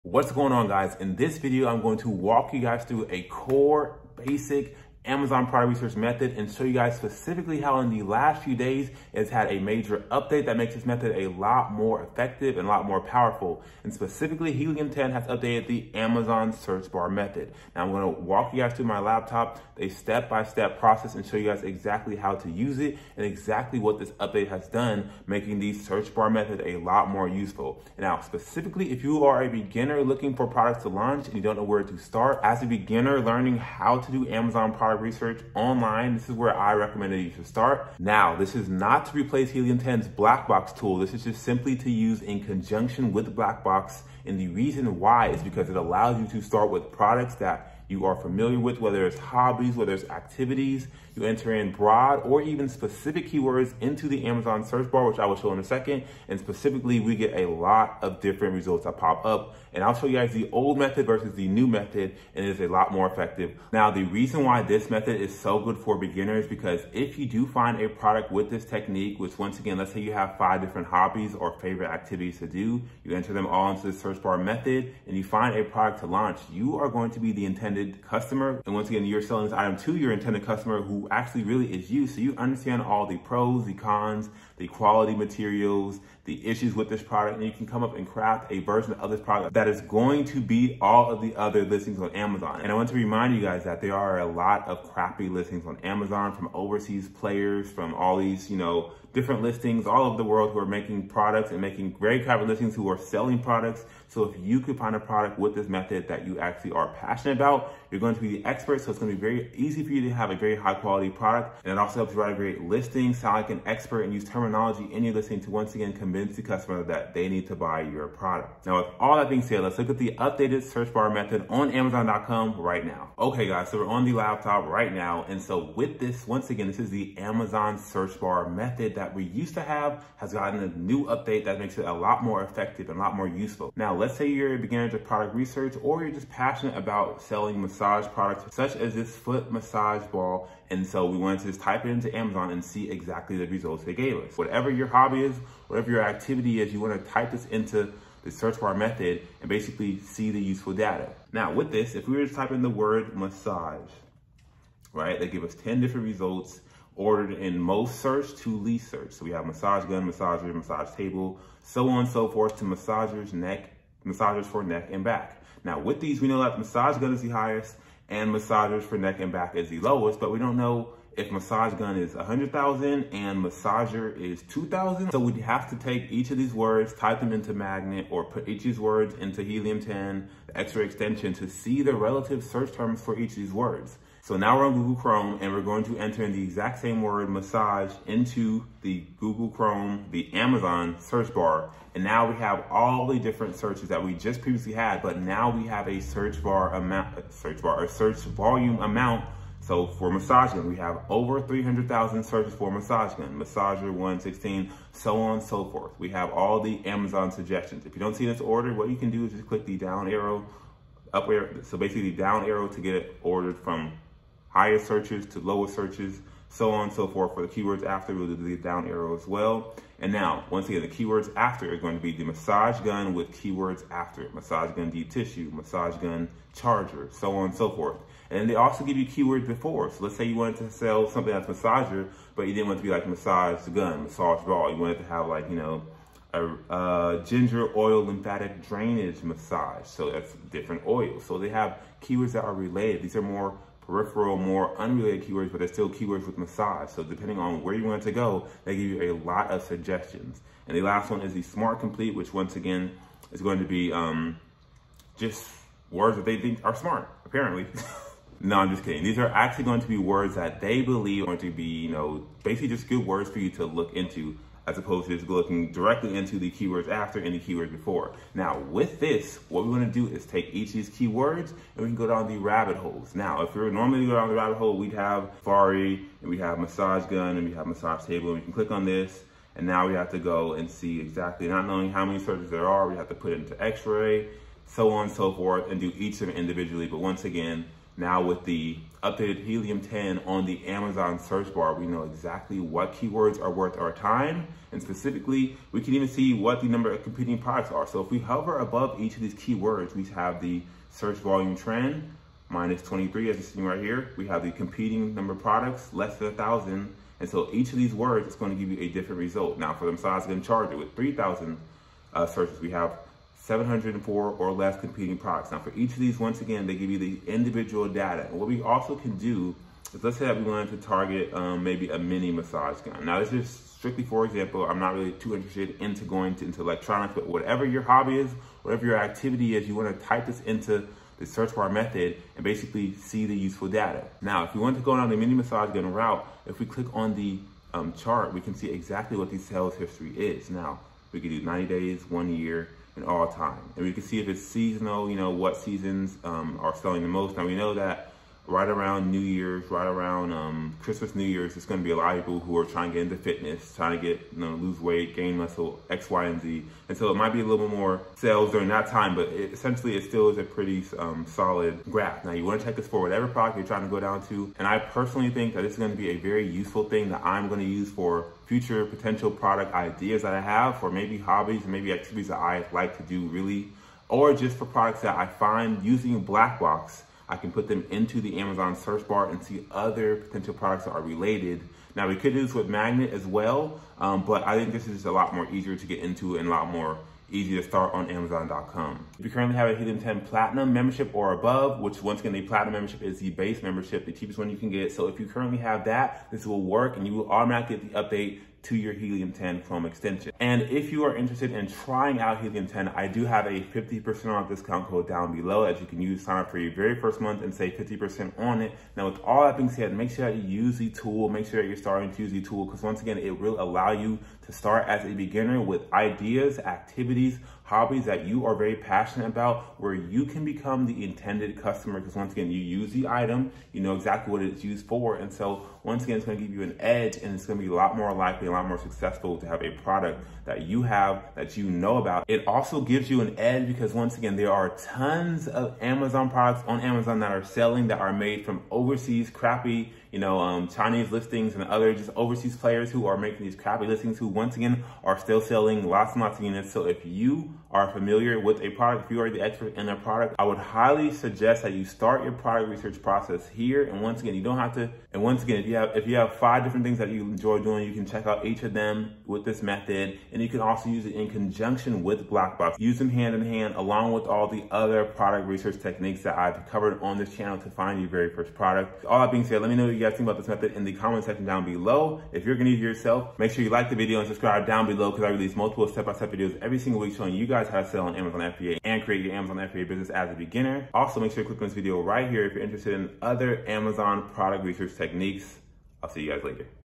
New Amazon FBA Product Research Method - Helium 10 Update Changes Product & Keyword Research!!
Amazon Product Research and Keyword Research are essential in finding products that have demand. Only products that are getting searches on a regular basis will be profitable and sustainable. In this video i go over a Helium update to amazons search bar. With this amazon product research method we are able to find products much more efficiently due to having more data right on amazon.

Time Codes

0:00 - Introduction
2:03 - Amazon Product Research Method
3:04 - Helium 10 extension update
5:39 - Helium 10 Product Research Validation
6:40 - Helium 10 Discount Code

 helium 10 tutorial amazon keyword research